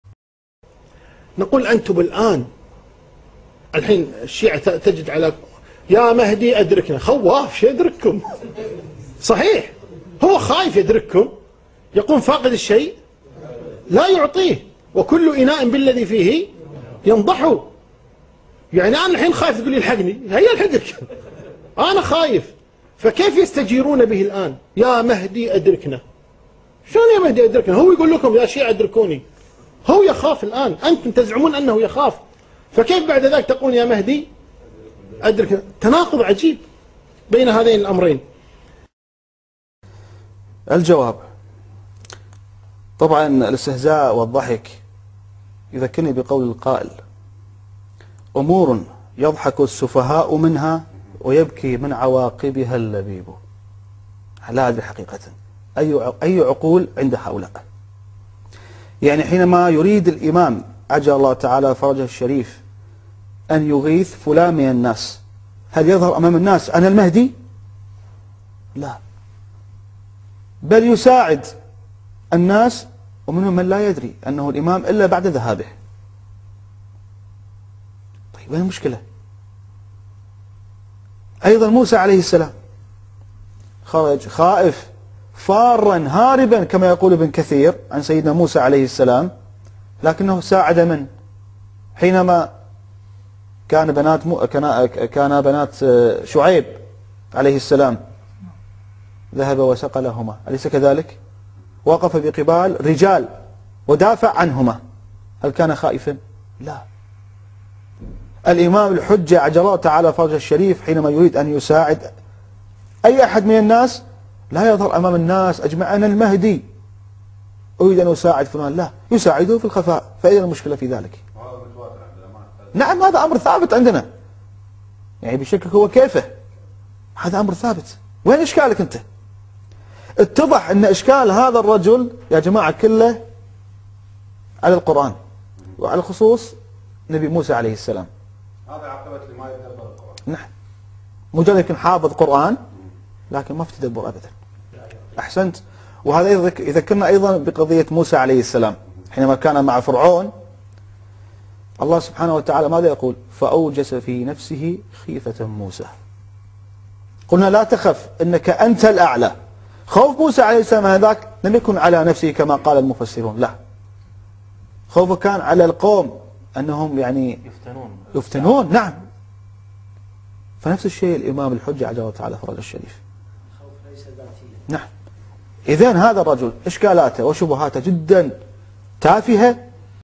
هل يستطيع مهدي الشيعة مساعدة أحد منهم ؟
محامي أهل البيت - شبكة راصد
الرد على الوهابي الشيخ عثمان الخميس بخصوص خروج الامام المهدي من قبل محامي اهل البيت